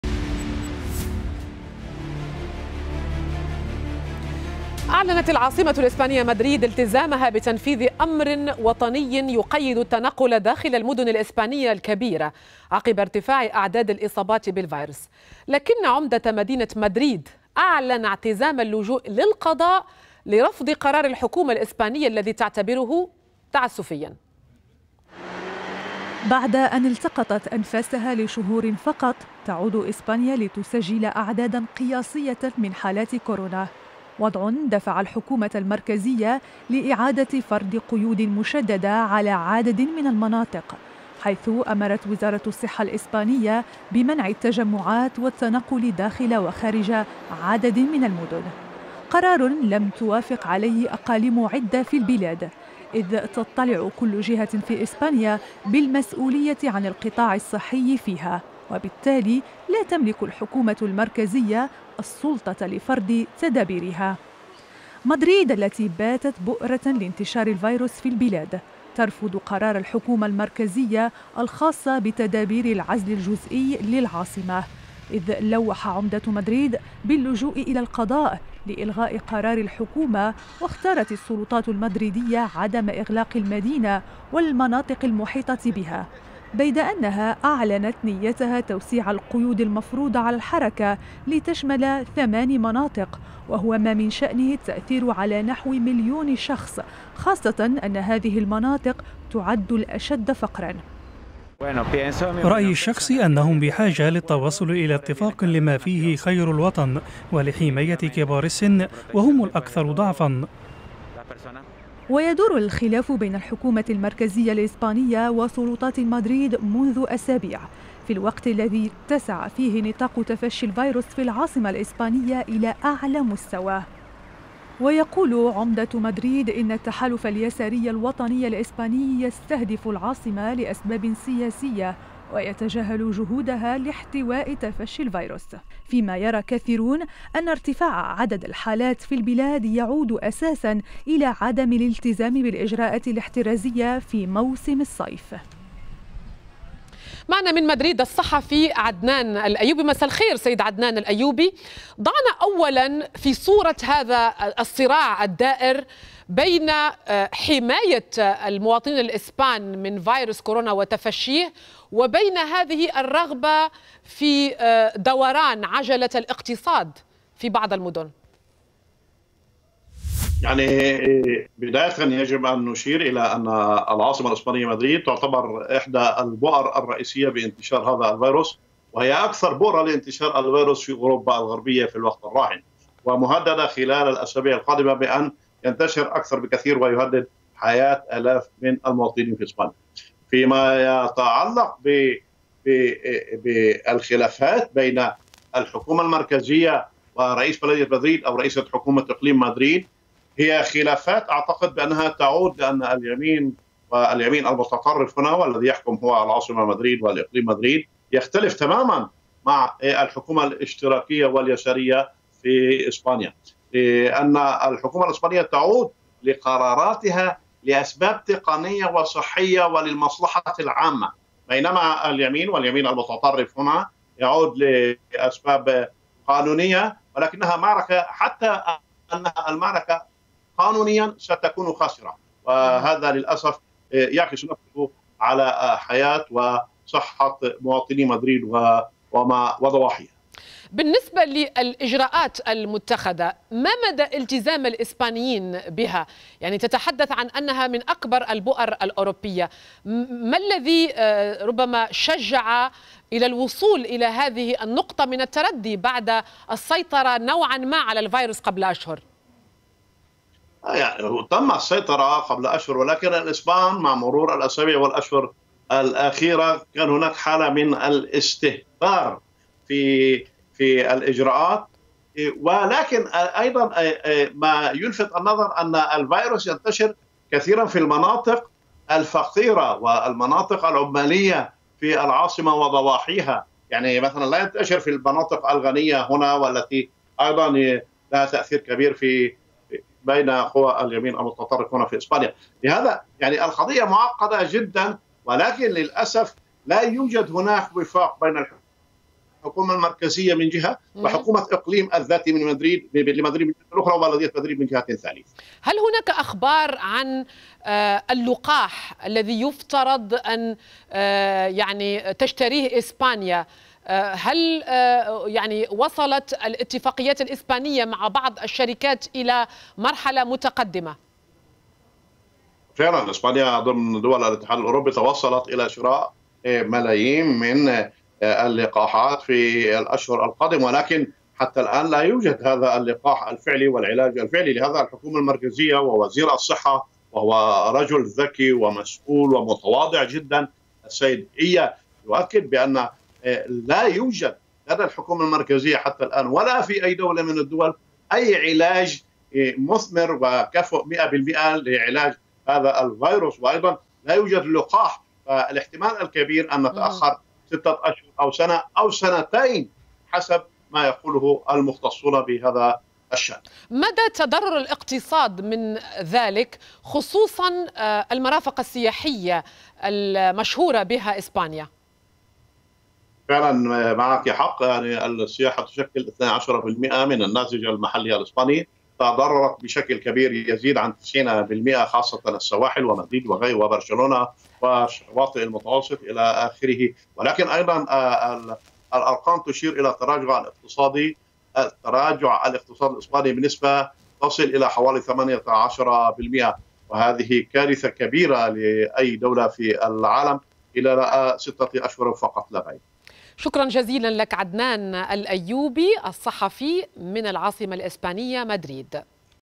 إسبانيا.. الحكومة المركزية تدعو مدريد إلى تشديد قيود كورونا
أعلنت العاصمة الإسبانية مدريد التزامها بتنفيذ أمر وطني يقيّد التنقل داخل المدن الإسبانية الكبيرة، عقب ارتفاع أعداد الإصابات بفيروس كورونا.
لكن عمدة مدينة مدريد أعلن اعتزام اللجوء للقضاء لرفض قرار الحكومة الإسبانية الذي تعتبره تعسفياً.
للمزيد من التفاصيل مع ضيف سكاي نيوز عربية من مدريد الصحفي عدنان الأيوبي.